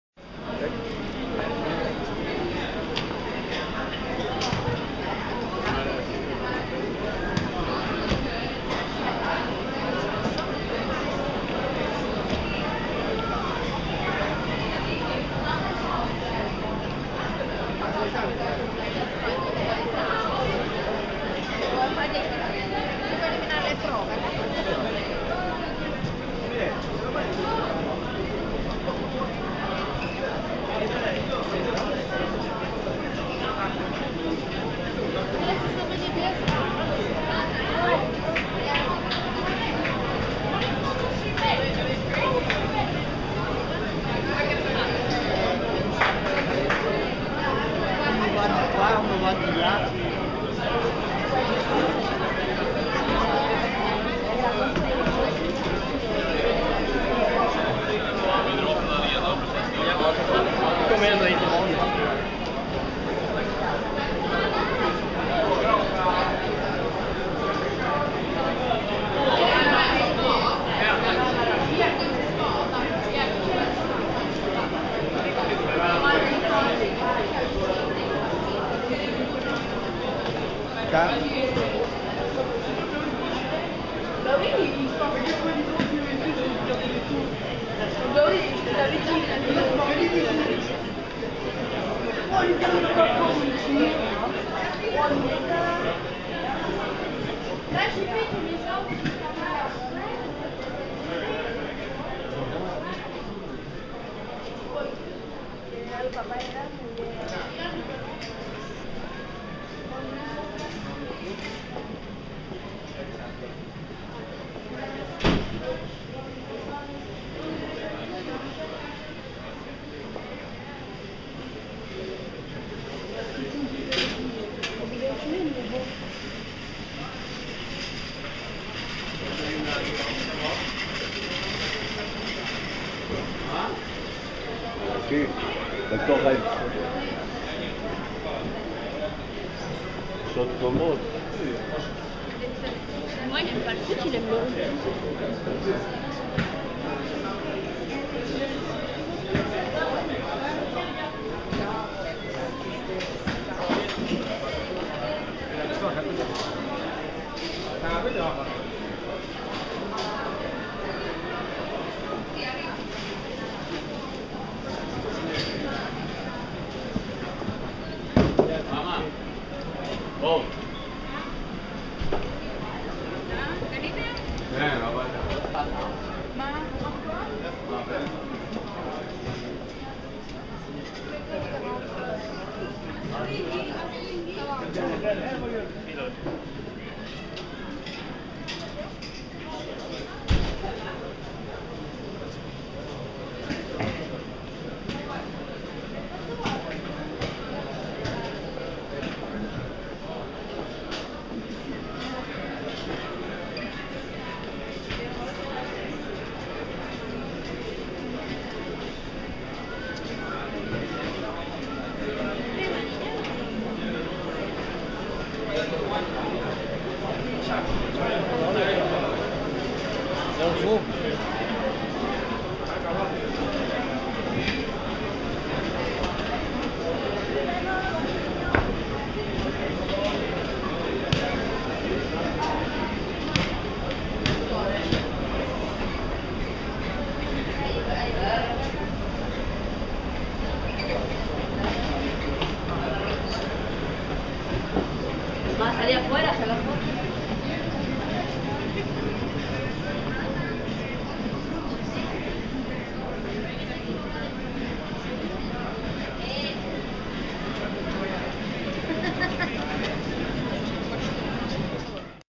The FC Barcelona Megastore located at Camp Nou
A walk through the giant FC Barcelona fanshop at Camp Nou.